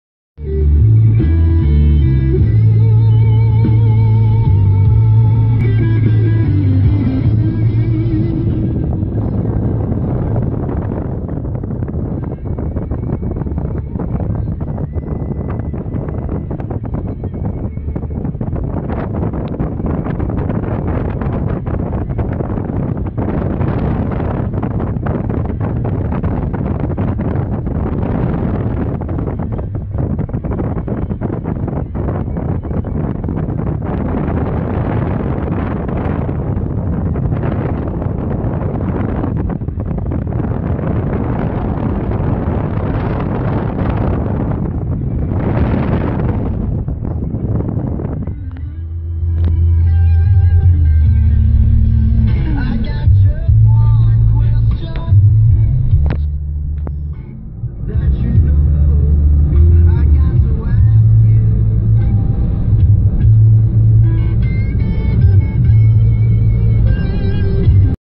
Johannesburg- William Nicol Drive looks like a cheap flea market with all the illegal advertising
Johannesburg- William Nicol Drive looks like a cheap flea market with all the illegal advertising trailers